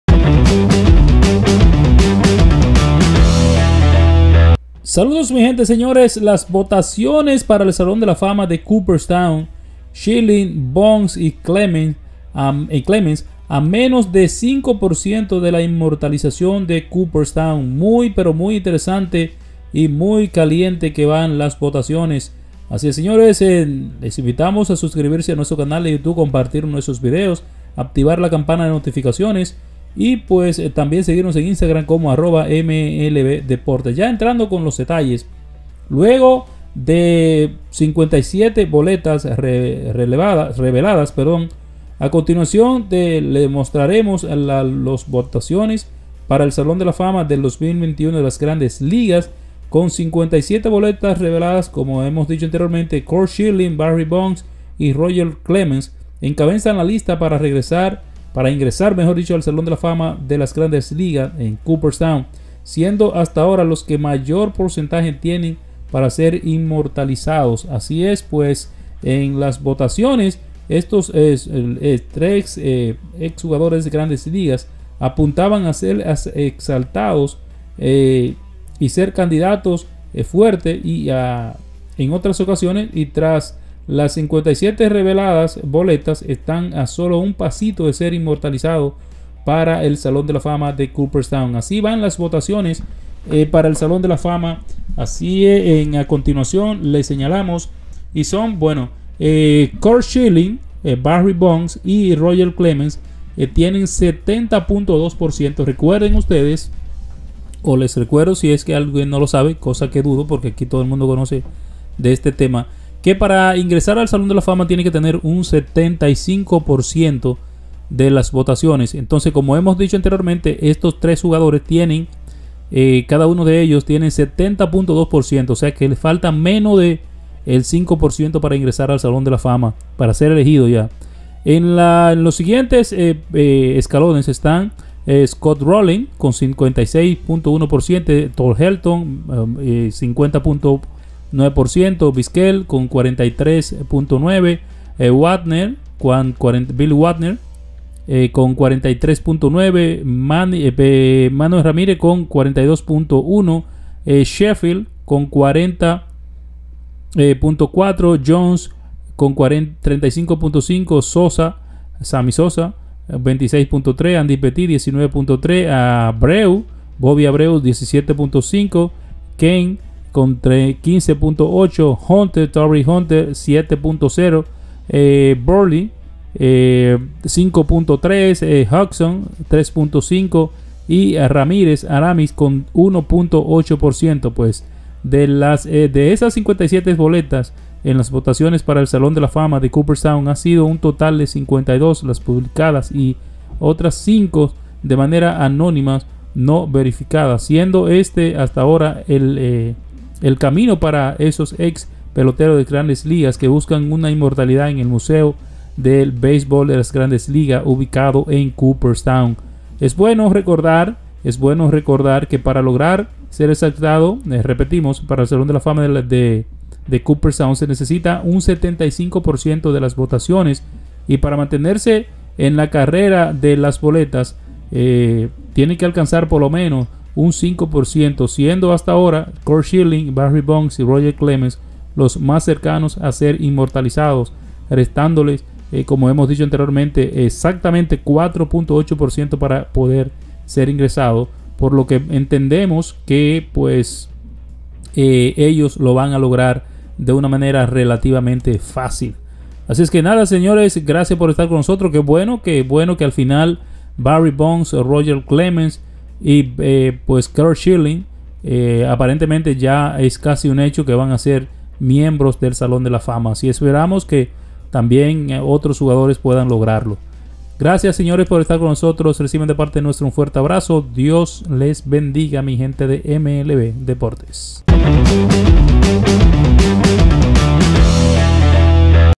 Schilling, Bonds y Clemens encabezan lista para ingresar al Salón de la Fama de las Grandes Ligas.
Luego de 57 boletas reveladas, a continuación te mostramos las votaciones para el Salón de la Fama 2021 de las Grandes Ligas (MLB).

Con 57 boletas reveladas en las votaciones,  Curt, Schilling Barry Bonds y Roger Clemens encabezan la lista para ingresar en el Salón de la Fama de las Grandes Ligas, siendo hasta ahora los que mayor porcentaje tienen para ser inmortalizados en Cooperstown.

En las votaciones estos los tres ex-peloteros apuntaban a ser altos candidatos y tras las 57 boletas reveladas, están a solo un pasito de ser inmortales en el Salón de la Fama de la MLB.

Así van las votaciones para el Salón de la Fama de la MLB:

Schilling Bonds y Clemens – 70.2%
Rolen – 56.1.%
Helton – 50.9%
Vizquel – 43.9%
Wagner – 43.9%
M. Ramírez – 42.1%
Sheffield – 40.4%
Jones – 35.1%
Sosa – 26.3%
Pettitte – 19.3%
Abreu – 17.5%
Kent – 15.8%
Hunter – 7.0%
Buehrle – 5.3%
Hudson – 3.5%
A.Ramírez – 1.8%
De esas 57 boletas en la votaciones para el Salón de Fama, han sido un total de 52 las públicas y otras cinco de manera anónimas/no verificables, siendo este hasta ahora el camino para esos ex-peloteros que buscan inmortales en el museo de la MLB ubicado en Cooperstown.

Es bueno recordar que para lograr ser exaltado al Salón de la Fama de MLB se necesita un 75% de las votaciones y para mantenerse en carrera en las boletas el 5%, siendo hasta ahora Curt, Schilling Barry Bonds y Roger Clemens los más cercano a ser inmortales, restándole 4.8% para lograrlo.